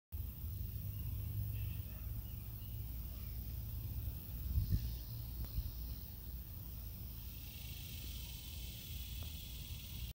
Cicada loading screen
he doesn't have his wings yet

this is my first time finding a cicada before it molts

gay rights